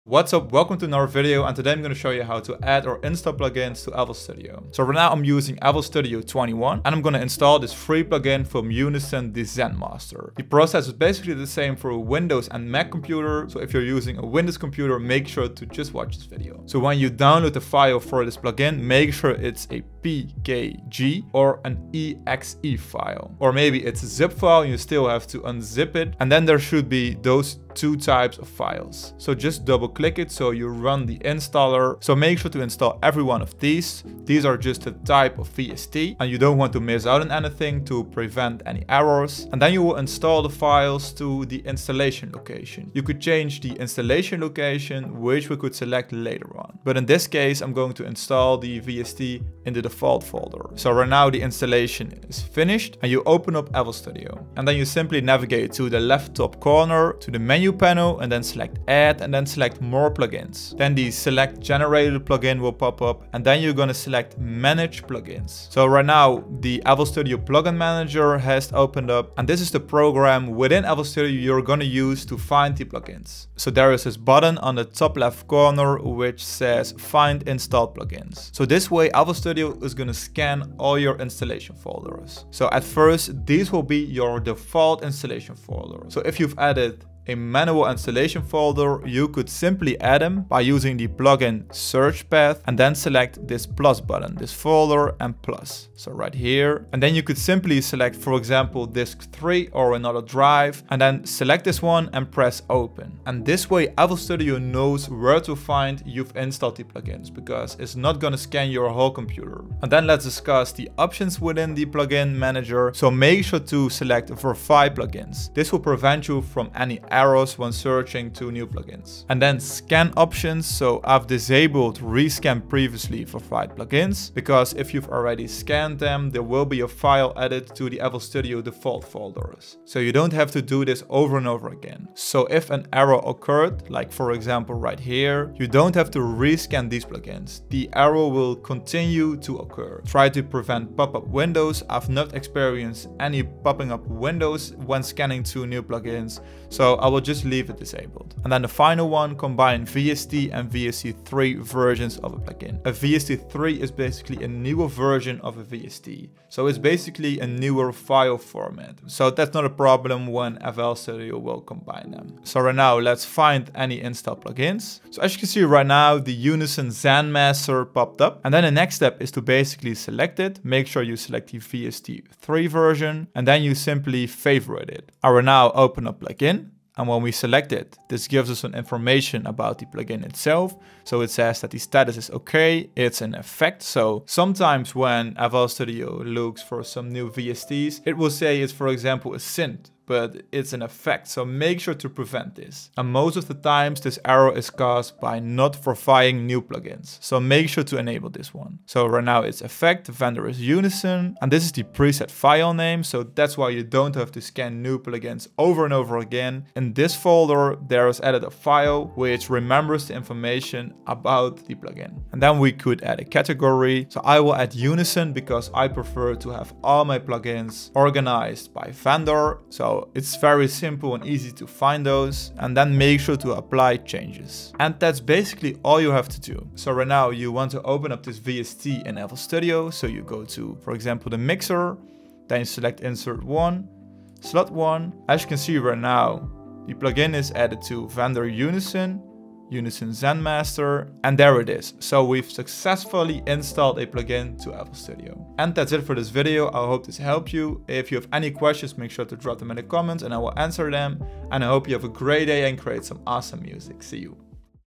How To Add Plugins To FL Studio 21 | How To Install VST Plugins
🎓 6 Steps For Making $100K a Year As a Producer - The Producer Business Formula

In this video I am going to show you how to How To Add Plugins To FL Studio 21 or How To Install VST Plugins.